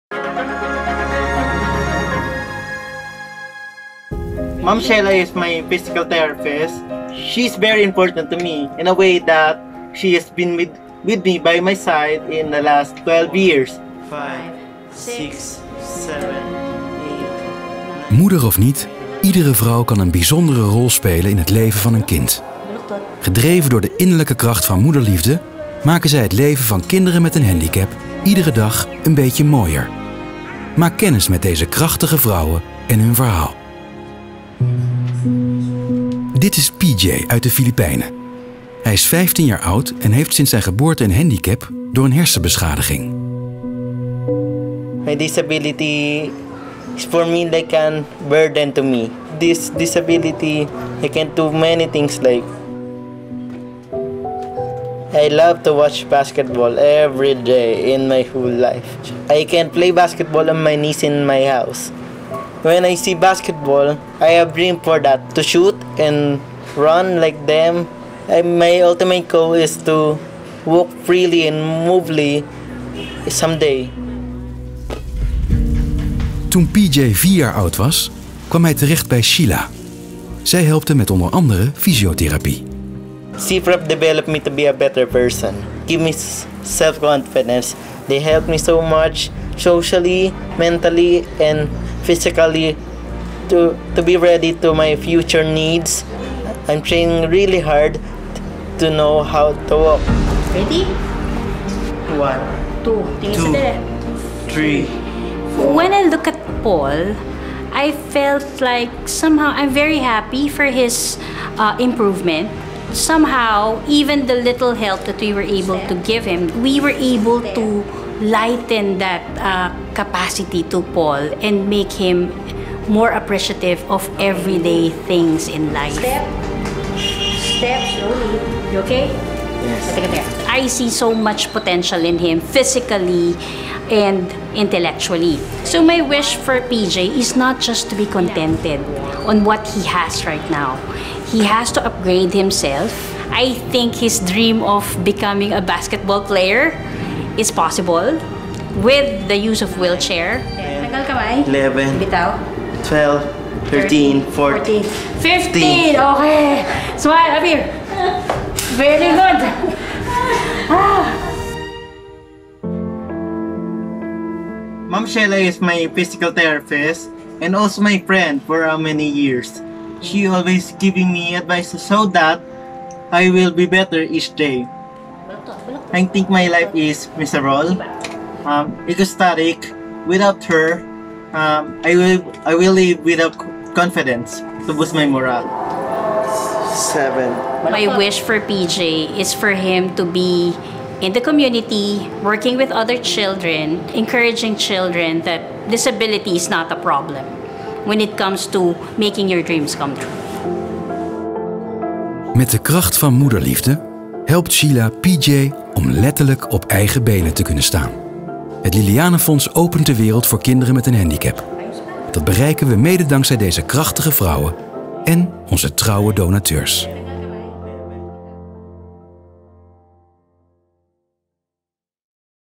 Geen berg te hoog voor PJ
PJ heeft een hersenbeschadiging. Dankzij Shella durft hij te dromen.